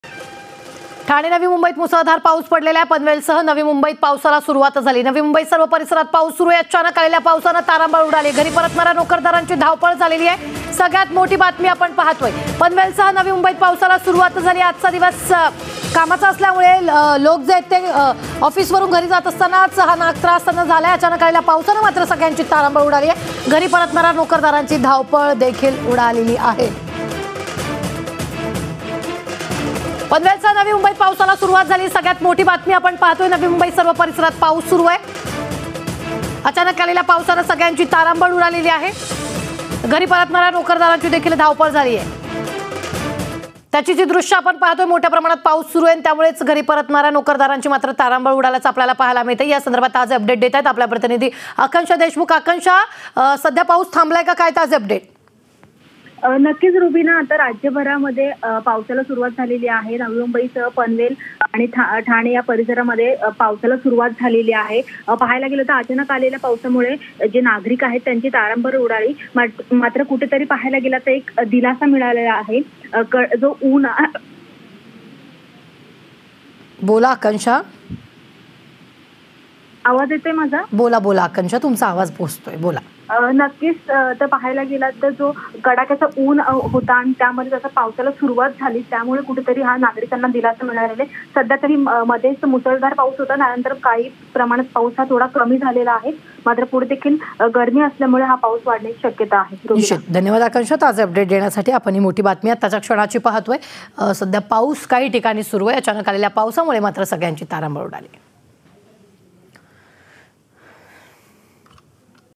Navi Mumbai Rain Update | नवी मुंबईत मुसळधार पाऊस, अचानक आलेल्या पावसानं नागरिकांंमध्ये तारांबळ
Credit: Political |snehal jagtap/Producer| Vaibhaw/Editor